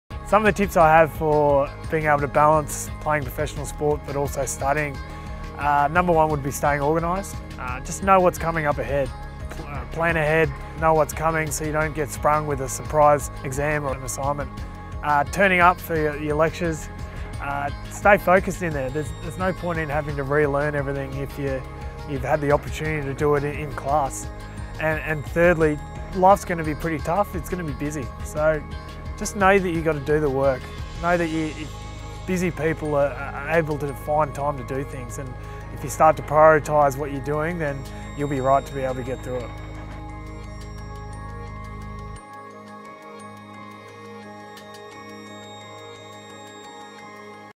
Nick Phipps shares his top tips for balancing sport and study
ISCA Ambassador and 70+ Test Wallaby Nick Phipps shares his top tips for finding the balance between playing sport and studying. Listen and learn from one of Australia's most successful rugby players about leadership, professional sports & achieving the balance between study & sport.

"Number one would be staying organised. Plan ahead, know what's coming up so you don't get sprung with a surprise exam or assignment".

Find out more about ISCA and where it can take you,